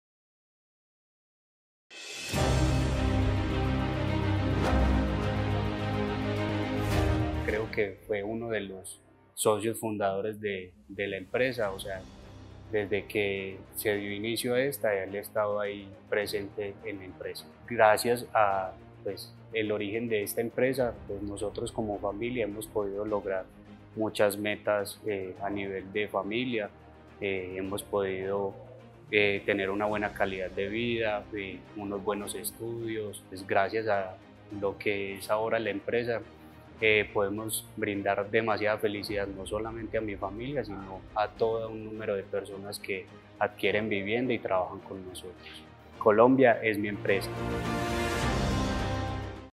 Conoce el héroe de Juan Camilo
Dejar huella es el superpoder del héroe de Juan Camilo. En el pasado su papá creó una empresa que hoy brinda a sus empleados un futuro con posibilidades. Las empresas colombianas crecen por su gente y generan oportunidades para todos. para conocer más.